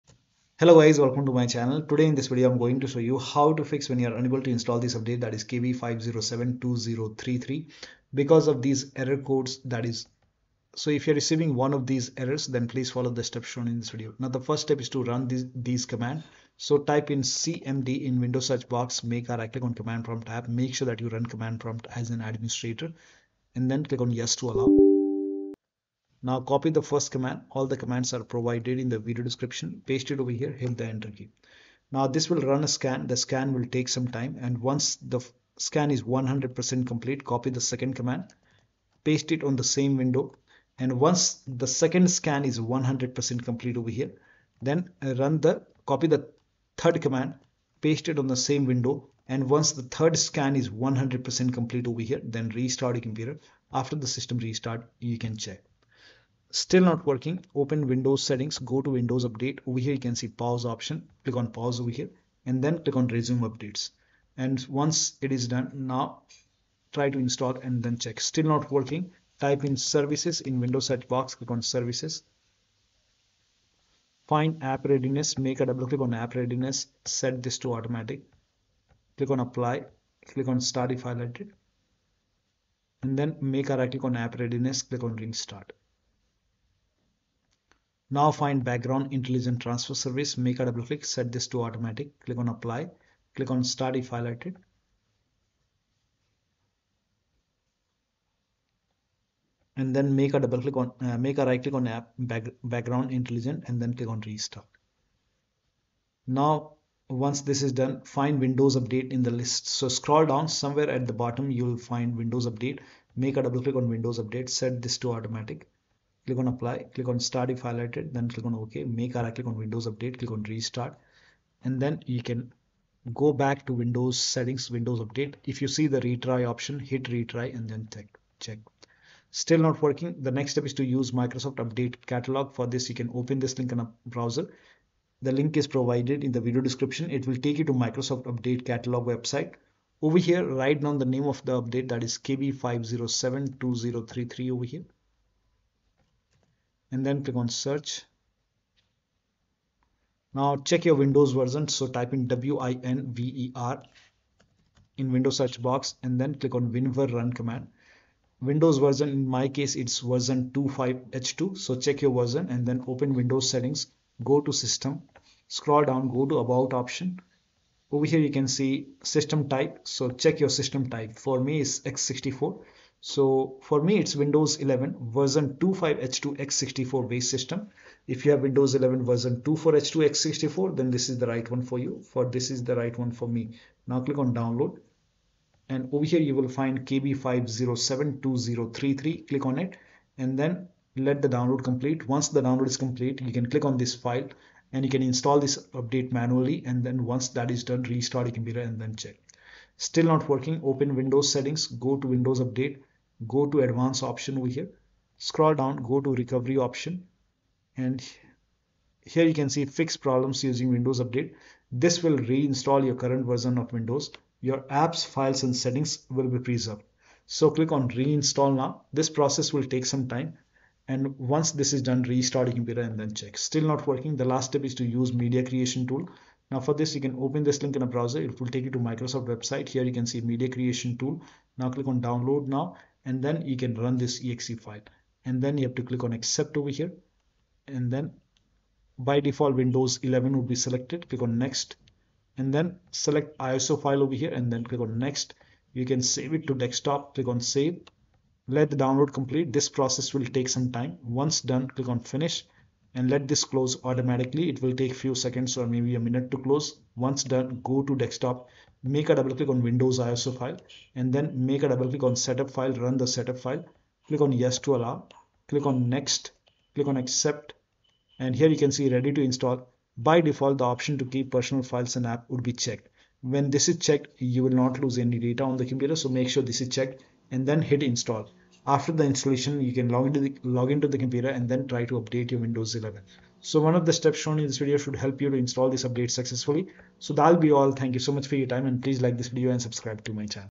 Fix Update KB5072033 Not Installing Error Code 0x800f0991/0x800f0831/0x80073712 Windows 11
How To Fix Update KB5072033 Not Installing Error Code 0x80073712/0x800f0991/0x800f0831 Windows 11